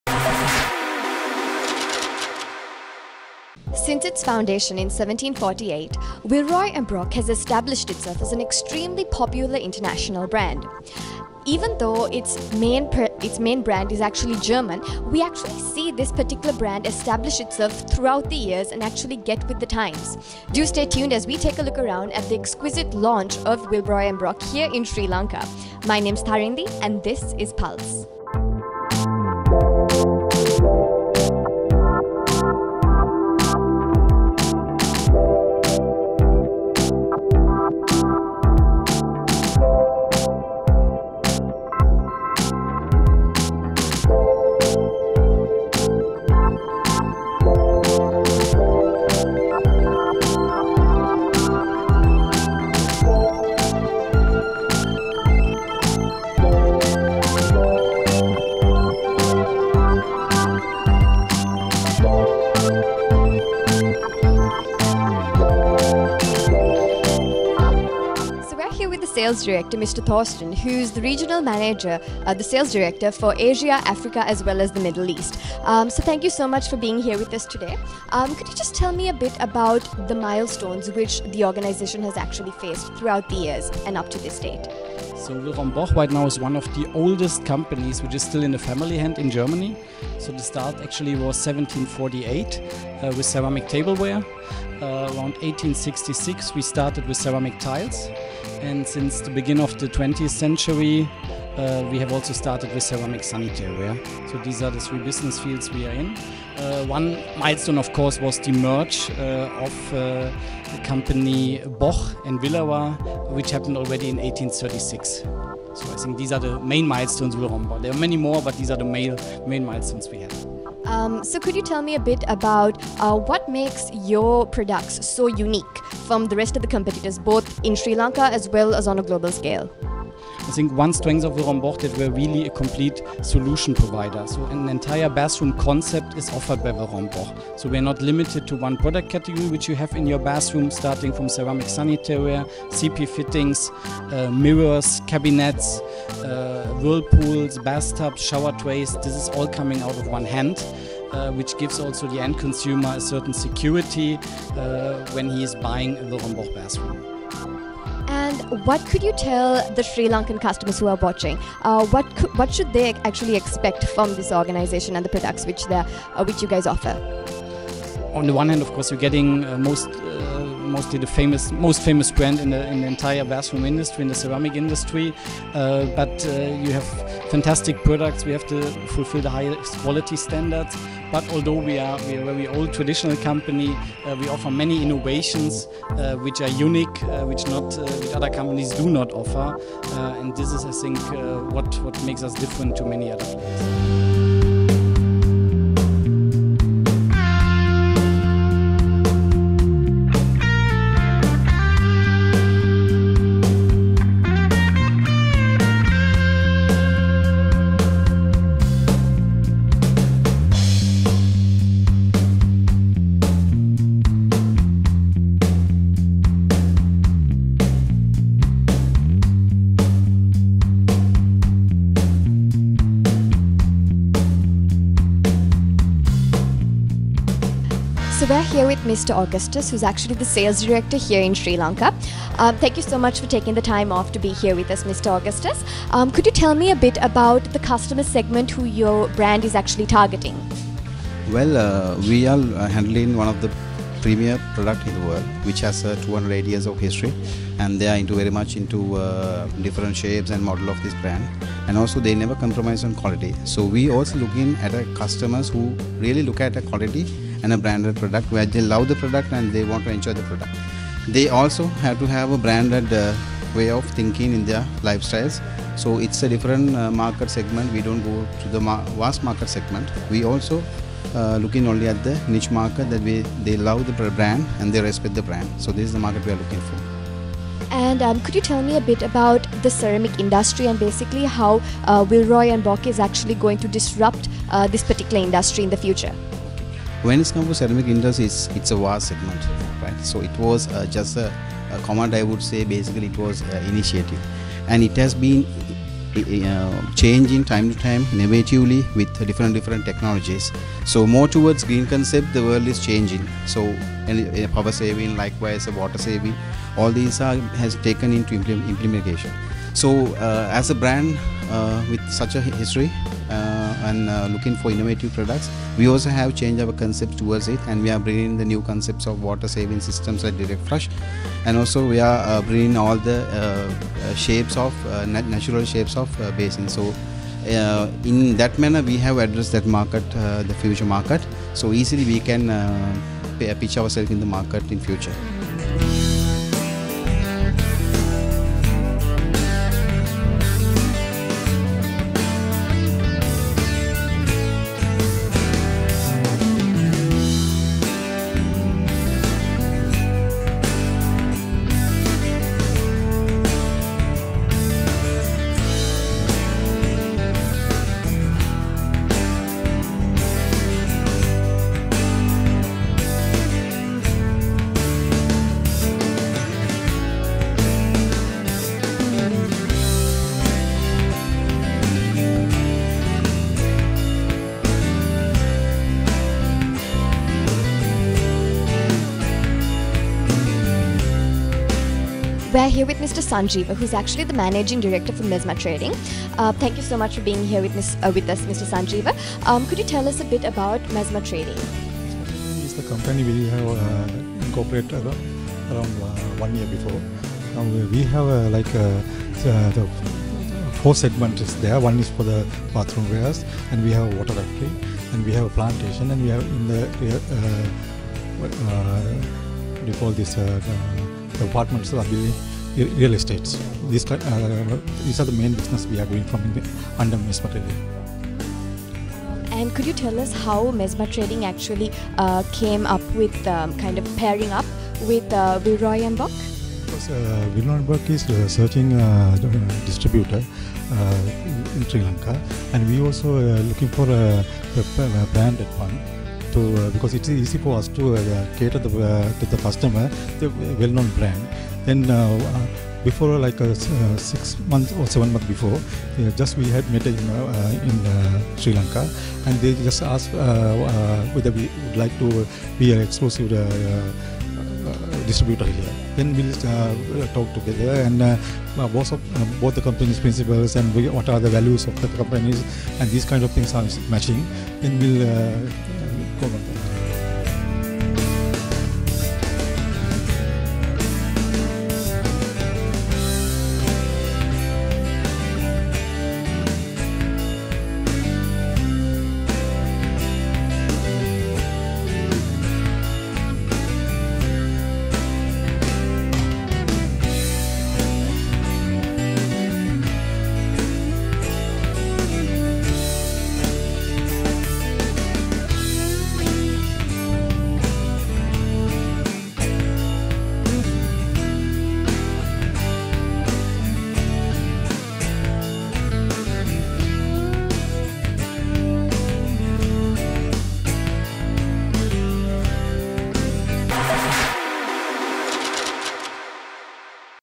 Launch of Villeroy and Boch in Sri Lanka
The world renowned German ceramics brand Villeroy & Boch officially launched in Sri Lanka with Mezma Trading Lanka (Private) Limited as their official representative!

Check out how it went!